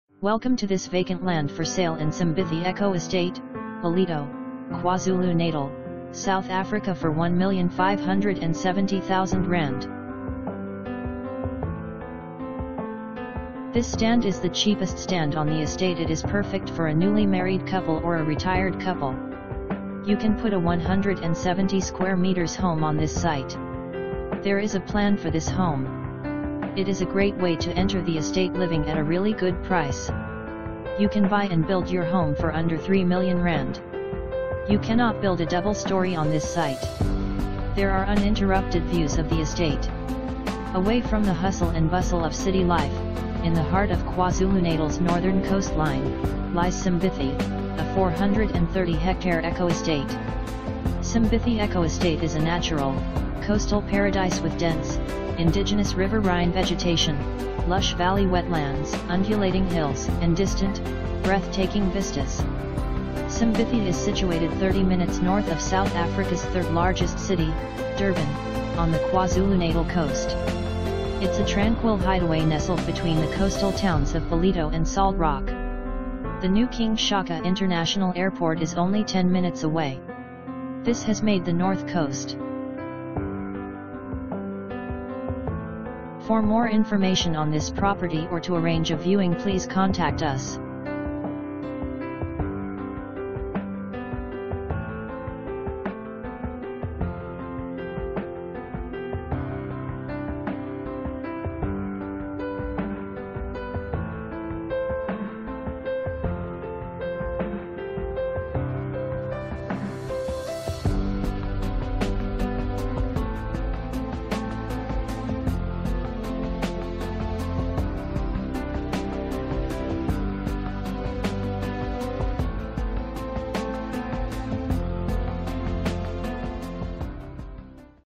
Vacant Land For Sale in Simbithi Eco Estate, Ballito, KwaZulu Natal, South Africa for ZAR 1,570,000
This stand is the cheapest stand on the estate,It is perfect for a newly married couple or a retired couple.You can put a 170 sq m home on this site.There is a plan for this home.It is a great way to enter the estate living at a really good price.You can buy and build your home for under 3 million rand.You cannot build a doub...